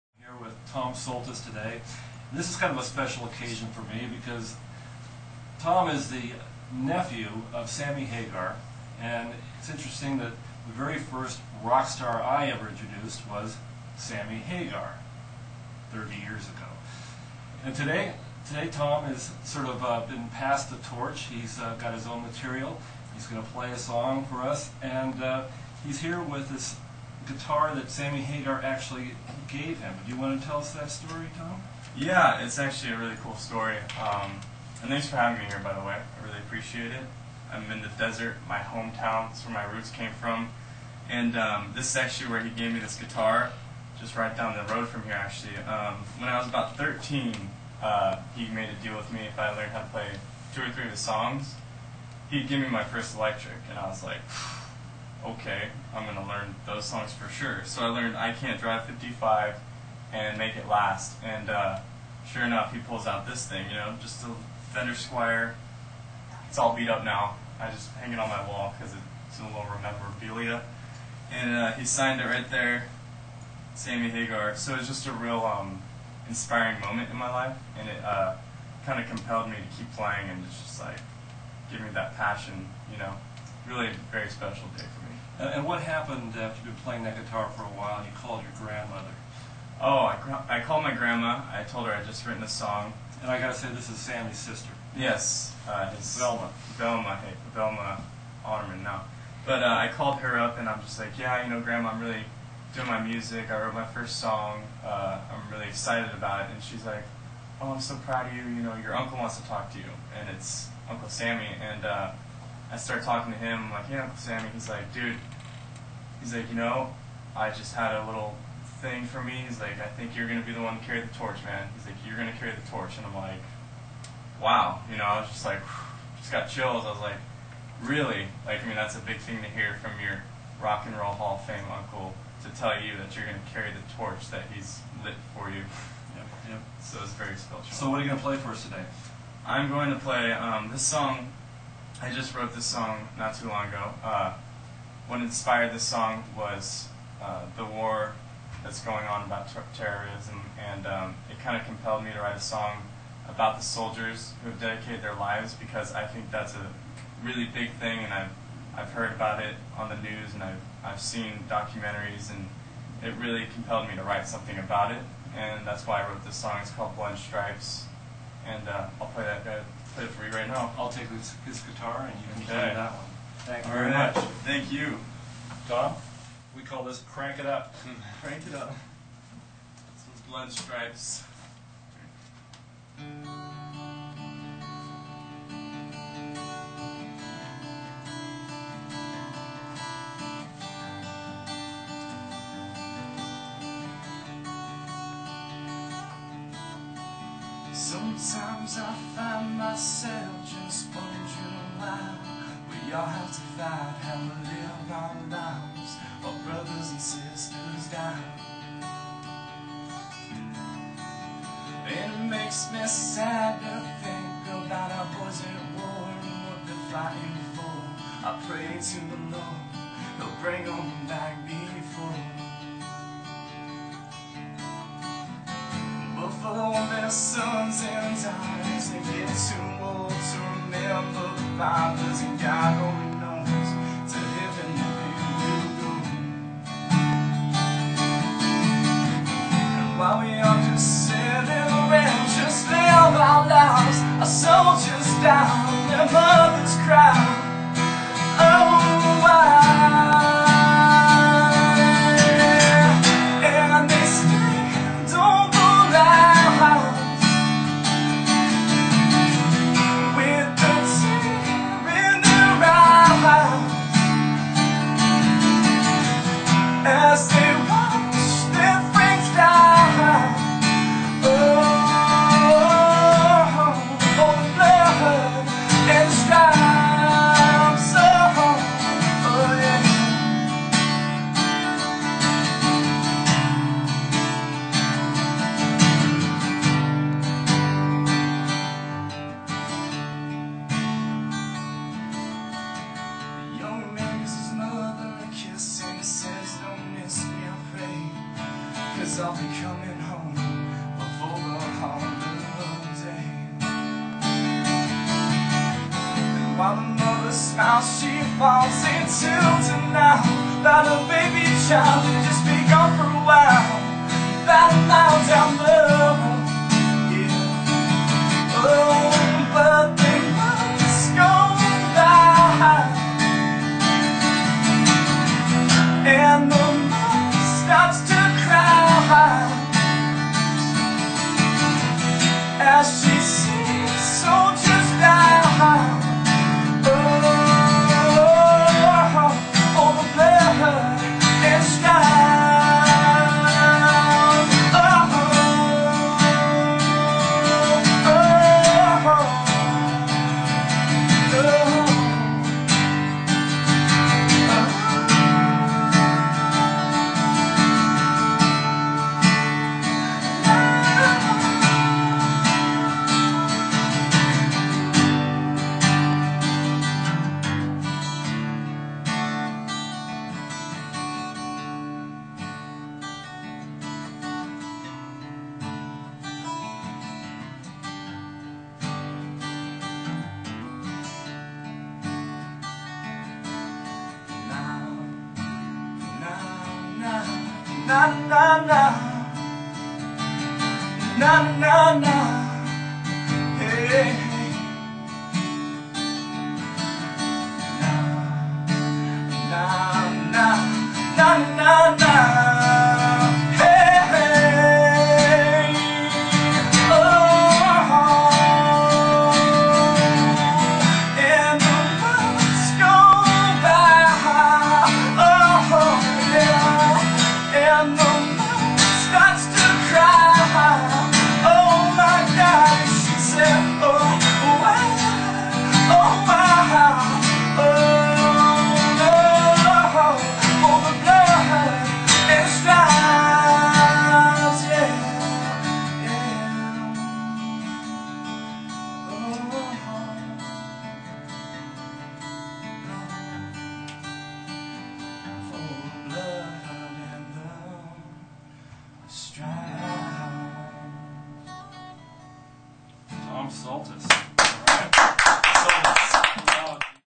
Crank it up with Tom Solis
Singer-guitarist Tom Solis and keyboard player Ronnie King crank it up in The Desert Sun studio